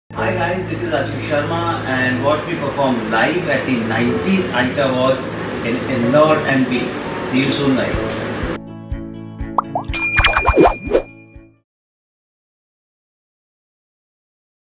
19th ITA Awards 2019 | Ashish Sharma Performing LIVE
Ashish Sharma Performing LIVE in The 19th ITA Awards.

सितारों भरी शाम, ज़बरदस्त लाइव परफॉरमेंस, 10 नवंबर को नेहरु स्टेडियम में... ऑनलाइन टिकट बुकिंग